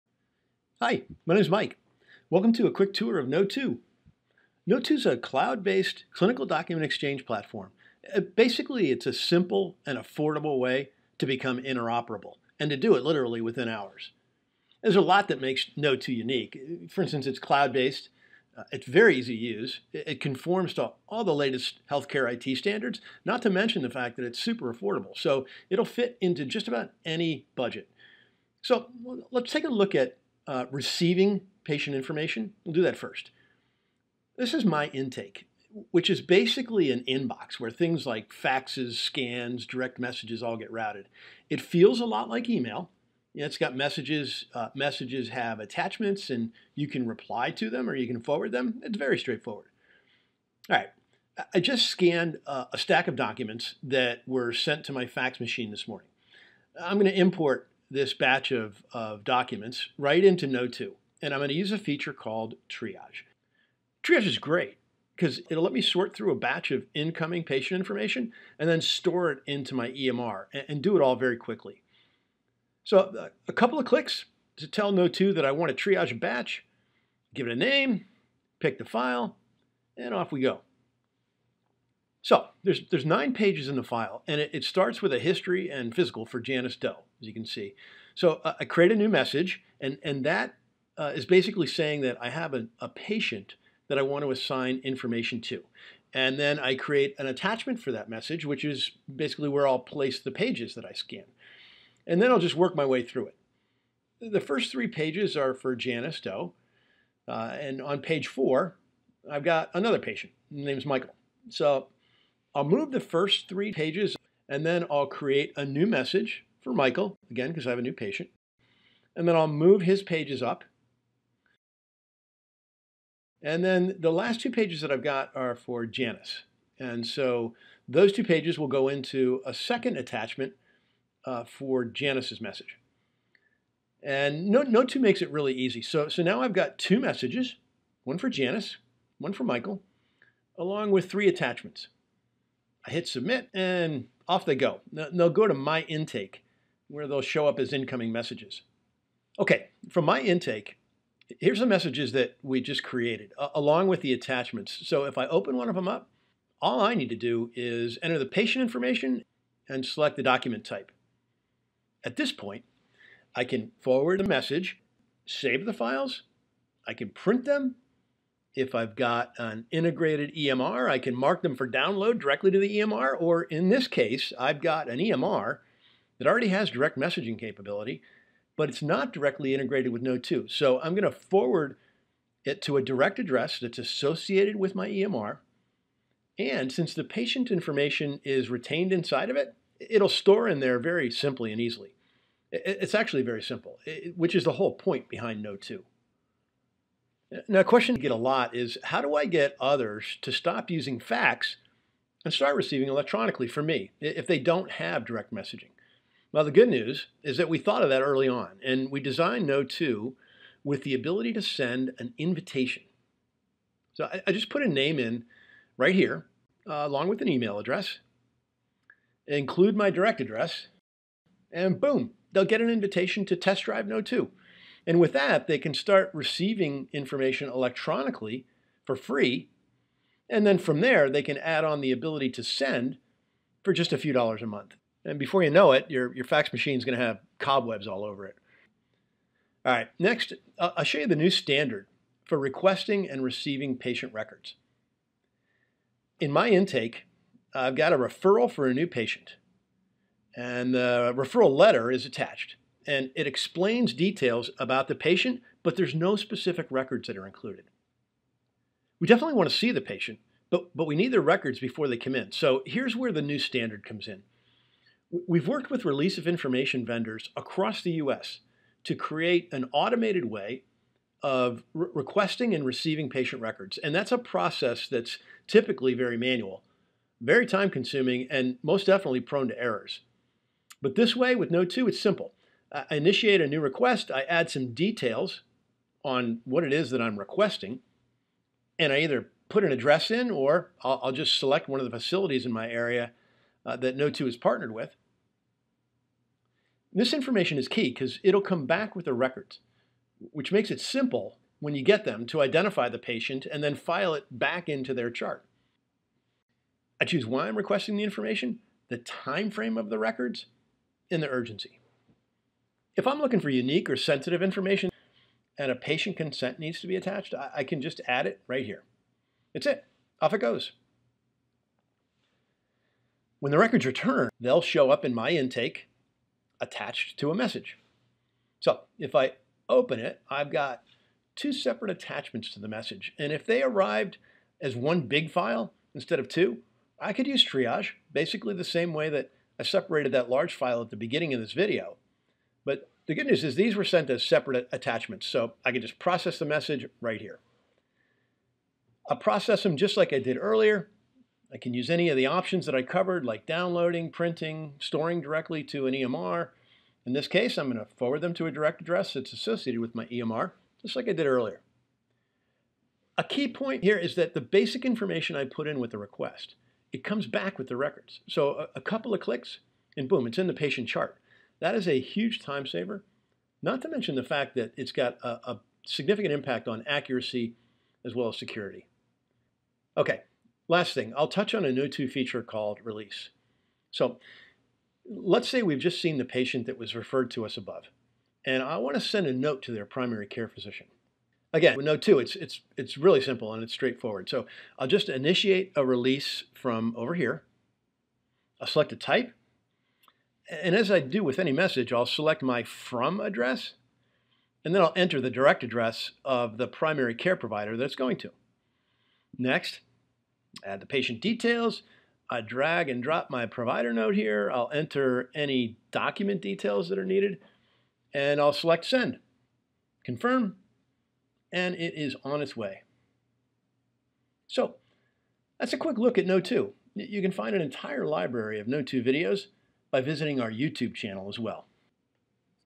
Kno2 Feature Overview
Take a quick tour of Kno2 to learn how this cloud-based clinical document exchange platform allows you to meet healthcare IT standards and quickly become interoperable. with all your healthcare workflows...all in one place.